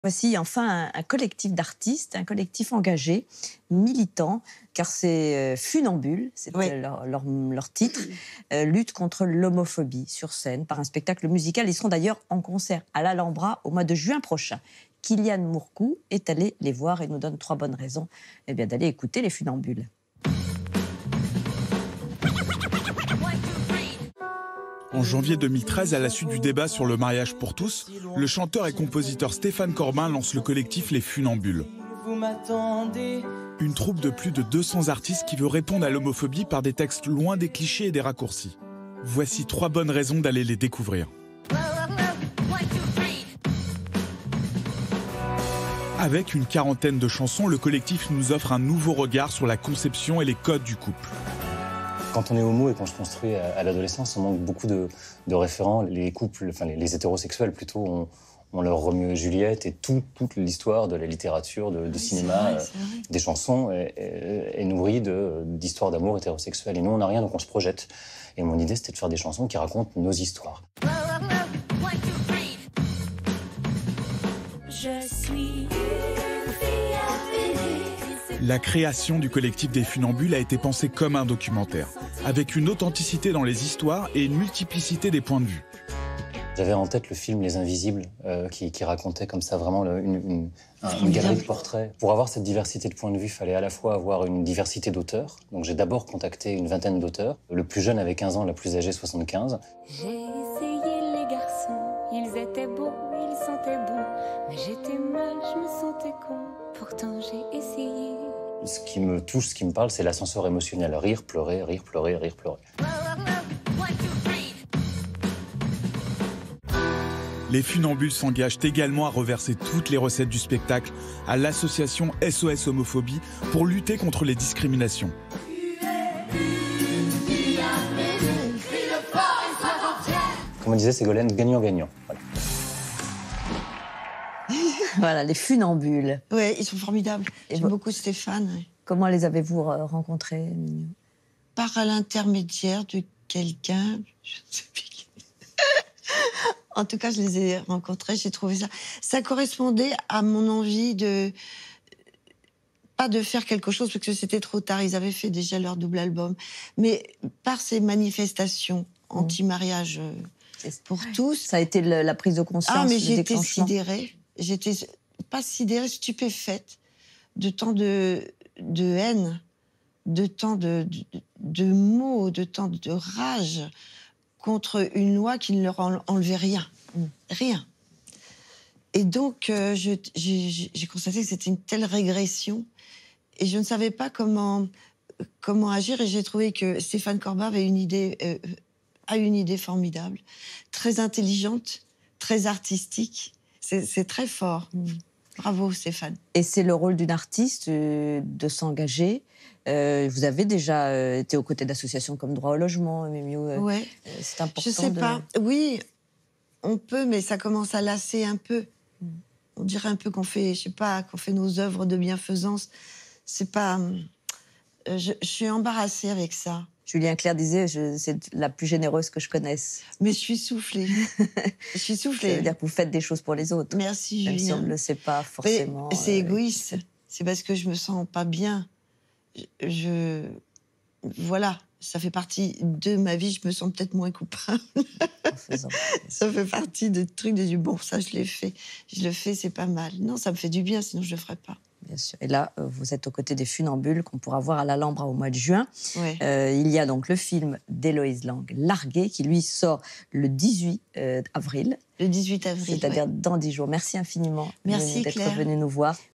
Trois bonnes raisons d'aller voir sur scène Les Funambules
De la musique pour témoigner de leur réalité et exprimer les obstacles, les espoirs et l'amour. Le collectif d'artistes, soutenu par la comédienne Miou-Miou, Les Funambules, luttant contre l’homophobie, est en concert sur la scène de l'Alhambra à Paris du 11 au 17 juin 2018.